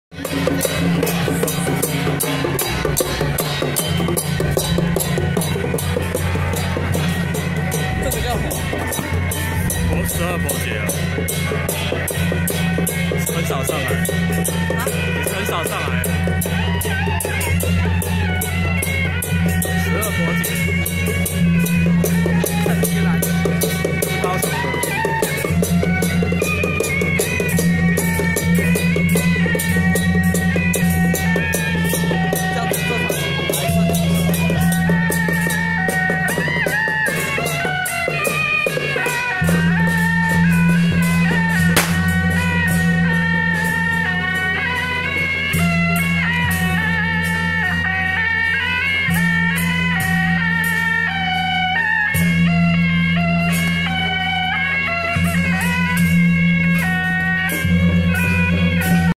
107新竹聖安宮開漳聖王民俗文化祭遶境【十二婆姐陣】
十二婆姐陣